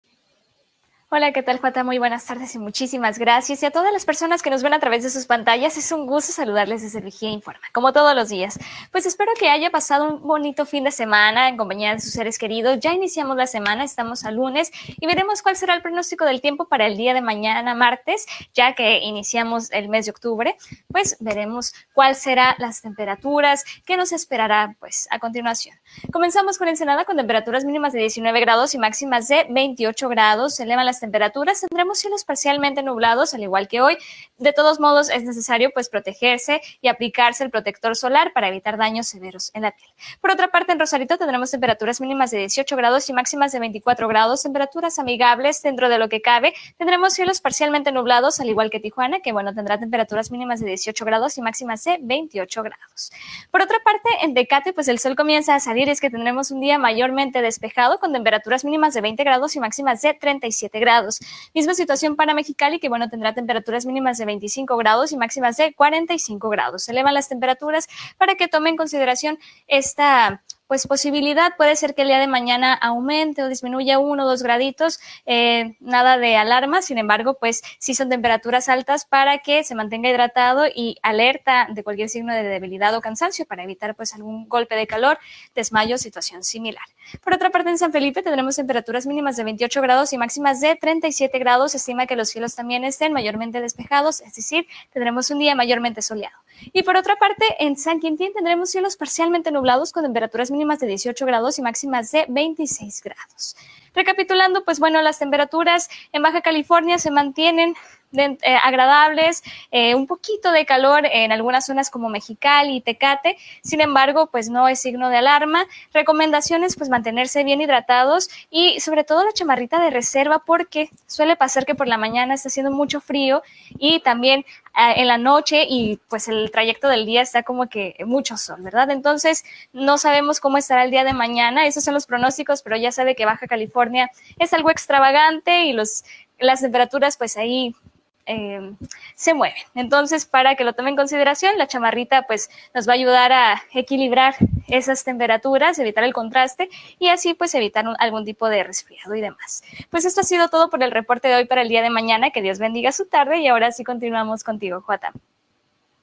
Pronóstico meteorológico para el martes 1 de octubre en Baja California
Alexandra Cañedo presenta el pronóstico del clima para los municipios de Baja California del día martes 1 de octubre.